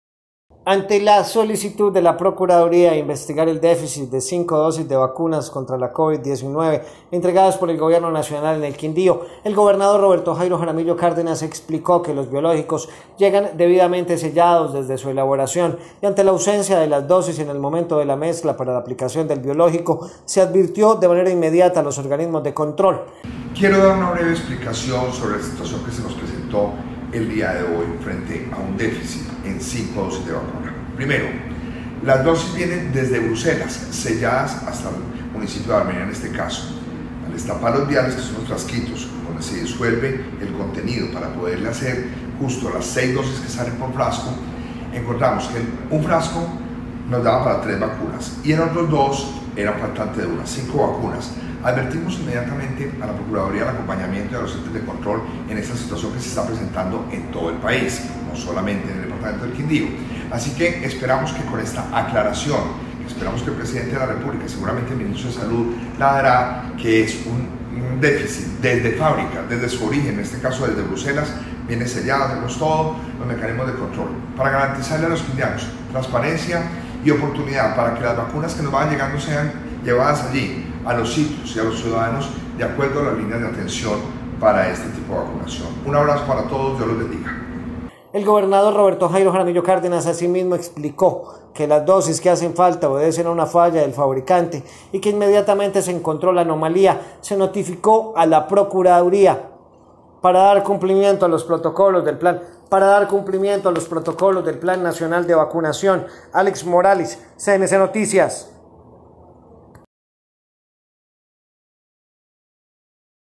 ¿Se perdieron?
Gobernador denunció  que faltan 5 dosis en la entrega de vacunas al Quindío.